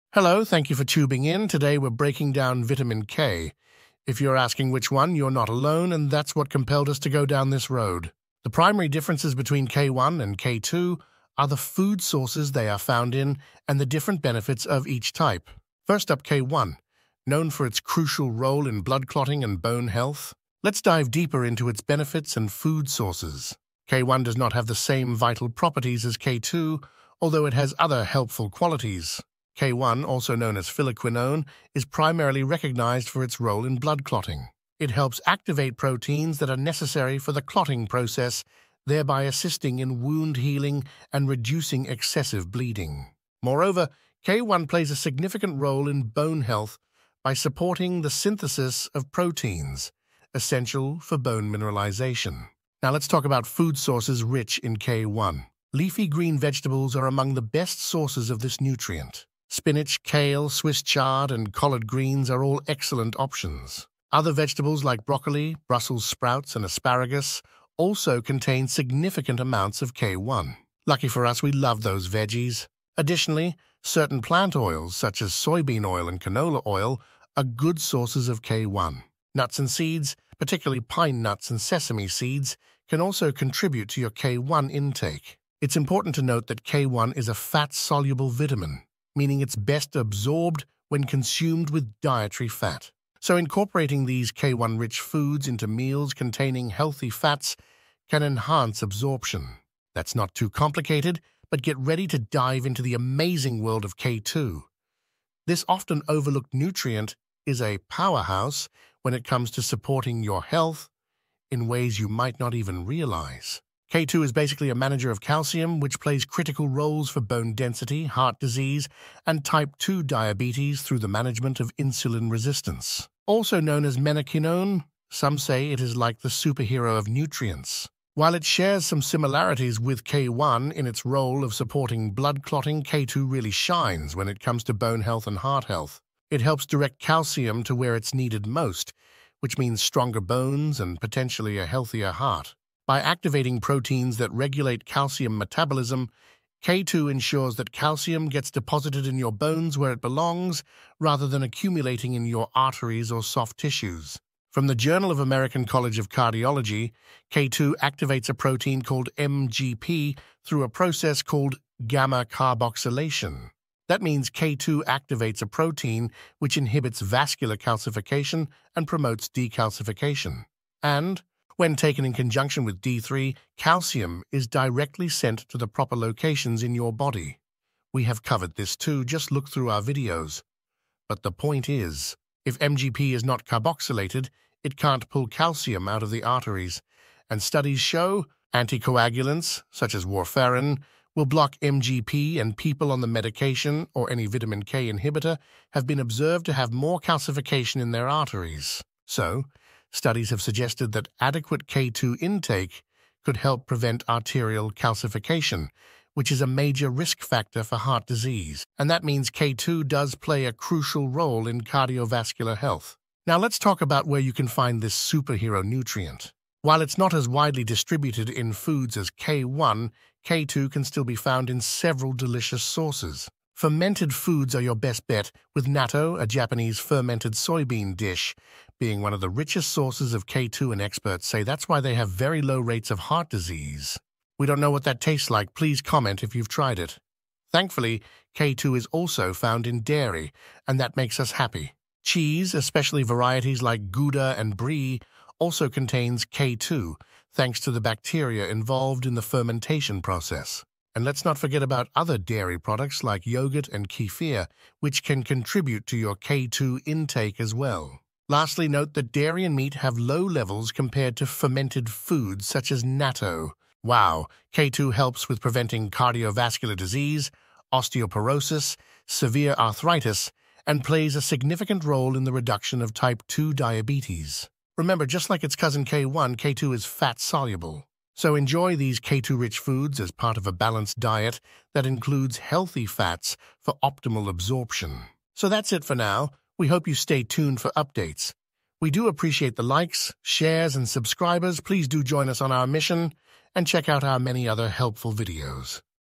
Why Vitamin K Is AMAZING For You!
Benefits of Vitamin K | Vitamin K Foods
⚡️Garden of Life Vitamin K2 and K1⚡️➜➜➜
Hello, thank you for tubing in, today we're breaking down Vitamin K. If you're asking which one, you're not alone, and that's what compelled us to go down this road. The primary differences between K1 and K2 are the food sources they are found in and the different benefits of each type.
First up K1, known for its crucial role in blood clotting and bone health, Let's dive deeper into its benefits and food sources. K1 does not have the same vital properties as K2, although it has other helpful qualities. K1, also known as phylloquinone, is primarily recognized for its role in blood clotting. It helps activate proteins that are necessary for the clotting process, thereby assisting in wound healing and reducing excessive bleeding. Moreover, K1 plays a significant role in bone health by supporting the synthesis of proteins essential for bone mineralization. Now, let's talk about food sources rich in K1. Leafy green vegetables are among the best sources of this nutrient. Spinach, kale, Swiss chard, and collard greens are all excellent options. Other vegetables like broccoli, Brussels sprouts, and asparagus also contain significant amounts of K1. Lucky for us, we love those veggies. Additionally, certain plant oils, such as soybean oil and canola oil, are good sources of K1. Nuts and seeds, particularly pine nuts and sesame seeds, can also contribute to your K1 intake. It's important to note that K1 is a fat-soluble vitamin, meaning it's best absorbed when consumed with dietary fat. So, incorporating these K1-rich foods into meals containing healthy fats can enhance absorption. That's not too complicated but get ready to dive into the amazing world of K2! This often overlooked nutrient is a powerhouse when it comes to supporting your health in ways you might not even realize. K2 is basically a manager of calcium which plays critical roles for bone density, heart disease, and type 2 diabetes through the management of insulin resistance. also known as menaquinone, some say it is like the superhero of nutrients.
While it shares some similarities with K1 in its role of supporting blood clotting, K2 really shines when it comes to bone health and heart health. It helps direct calcium to where it's needed most, which means stronger bones and potentially a healthier heart. By activating proteins that regulate calcium metabolism, K2 ensures that calcium gets deposited in your bones where it belongs, rather than accumulating in your arteries or soft tissues. From the Journal of American College of Cardiology: K2 activates a protein called MGP through a process called “gamma carboxylation”. That means K2 activates a protein which inhibits vascular calcification and promotes decalcification. And, when taken in conjunction with D3, calcium is directly sent to the proper locations in your body. We have covered this too, just look through our videos. But the point is If MGP is not carboxylated, it can’t pull calcium out of the arteries. And studies show, Anticoagulants, such as Warfarin, will block MGP and people on the medication (or any Vitamin K inhibitor) have been observed to have more calcification in their arteries. So, studies have suggested that adequate K2 intake could help prevent arterial calcification, which is a major risk factor for heart disease. And that means K2 does play a crucial role in cardiovascular health. Now, let's talk about where you can find this superhero nutrient. While it's not as widely distributed in foods as K1, K2 can still be found in several delicious sources. Fermented foods are your best bet, with natto, a Japanese fermented soybean dish, being one of the richest sources of K2 and experts say that's why they have very low rates of heart disease.
We don't know what that tastes like, please comment if you've tried it. Thankfully, K2 is also found in dairy, And that makes us happy! Cheese, especially varieties like Gouda and Brie, also contains K2, thanks to the bacteria involved in the fermentation process. And let's not forget about other dairy products like yogurt and kefir, which can contribute to your K2 intake as well.
Lastly, note that dairy and meat have low levels compared to fermented foods such as “Natto”.
Wow, K2 helps with preventing cardiovascular disease, osteoporosis, severe arthritis, and plays a significant role in the reduction of type 2 diabetes. Remember, just like its cousin K1, K2 is fat-soluble. We will always include our sources/references, please conduct your own research and let us know your experience in the comments.